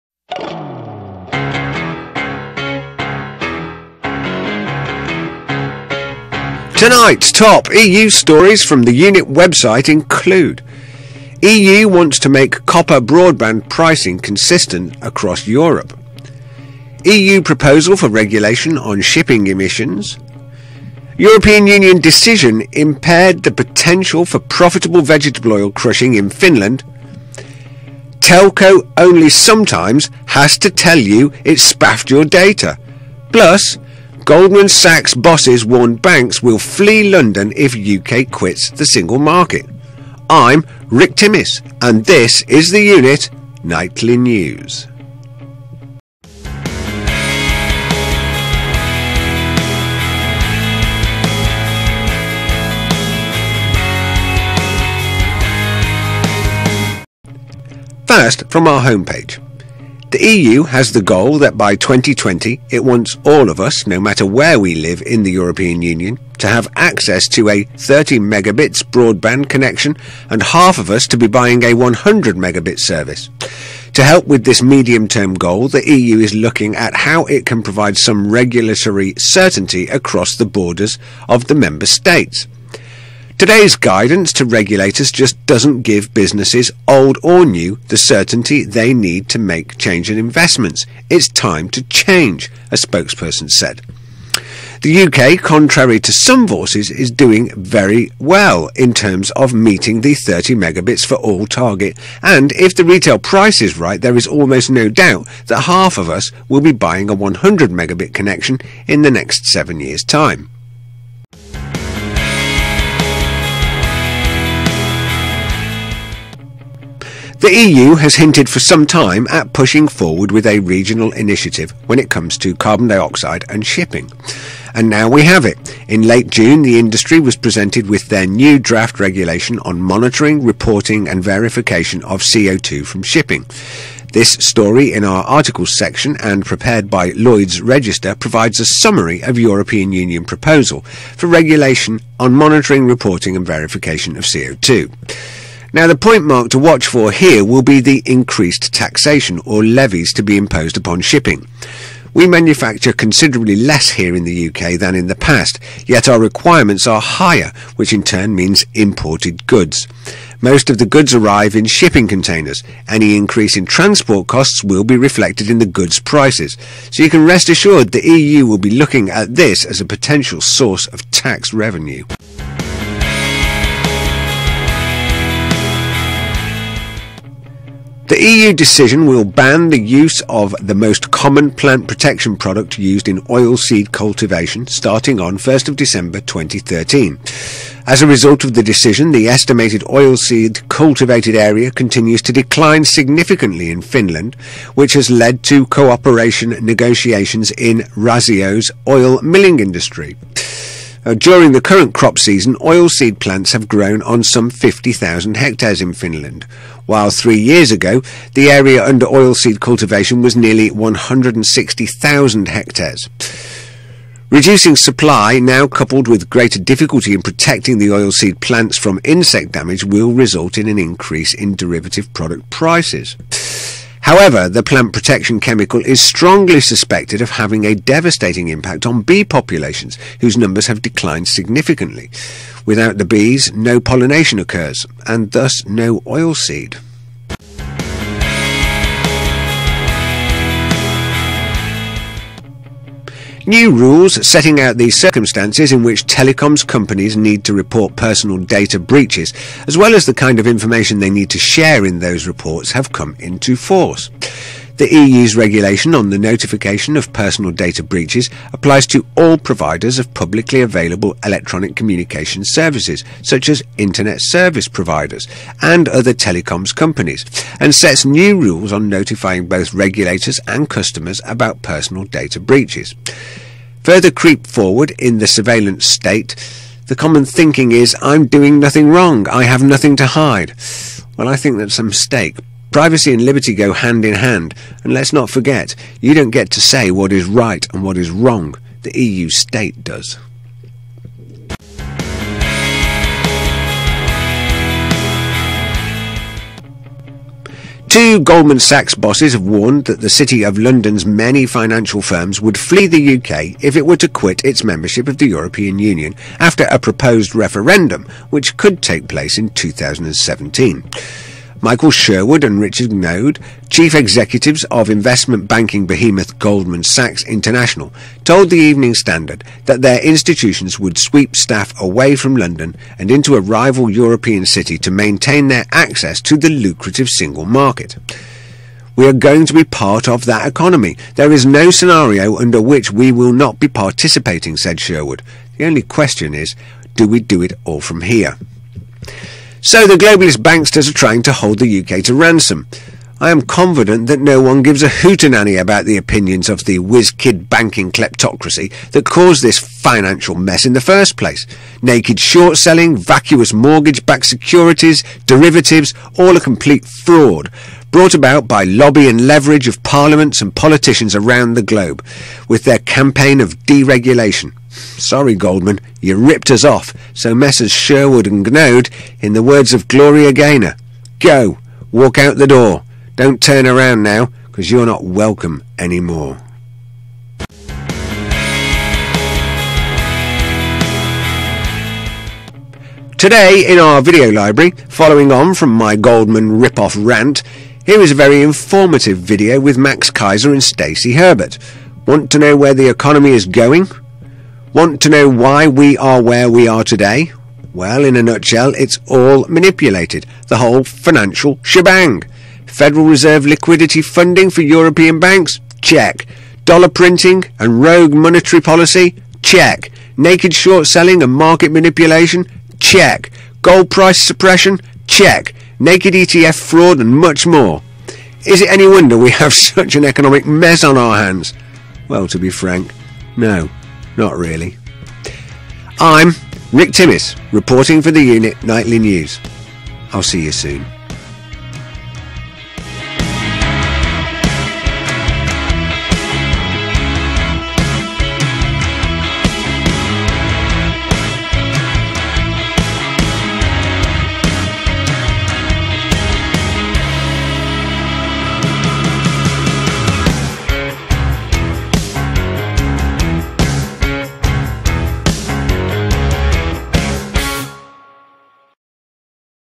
Fed Fraud - Goldman Goboff - Shipping Costs - Oil-Seed Rape
In today's stories:

1. EU wants to make copper broadband pricing consistent across EU

2. EU proposal for regulation on shipping emissions

3. EU decision impaired the potential for profitable vegetable oil crushing in Finland

4. Telco only SOMETIMES has to tell you it spaffed your data

5. Goldman Sachs Bosses Warn Banks Will Flee London if UK Quits Single Market